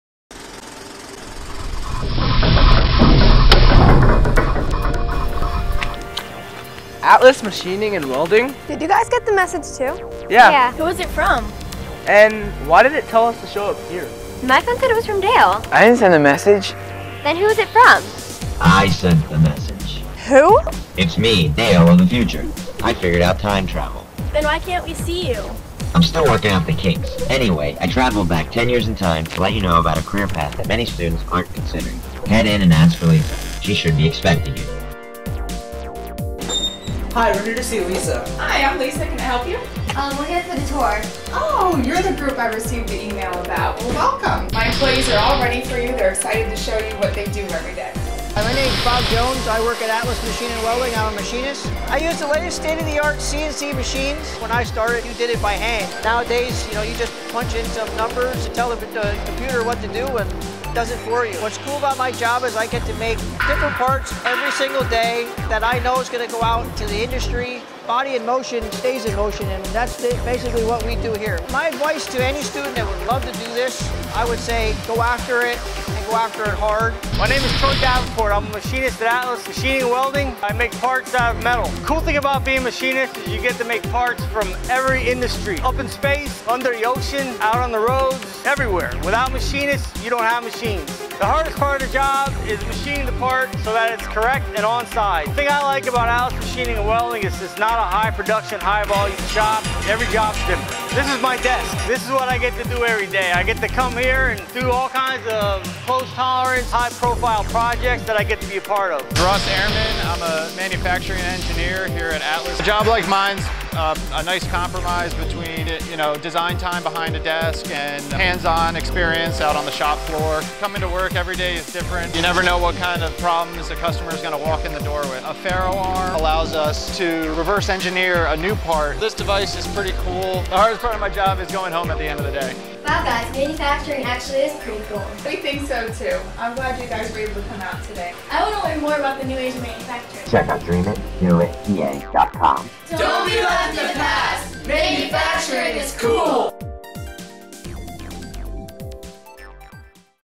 "What's So Cool About Manufacturing?" Atlas Machining & Welding, Inc. (2016)
Northern Lehigh Middle School's entry for Lehigh Valley Skills Up Partnership's "What's So Cool About Manufacturing?" student video contest.  NLMS received the Outstanding Videography Award for its work on a video for Atlas Machining & Welding, Inc. in Northampton, Pennsylvania.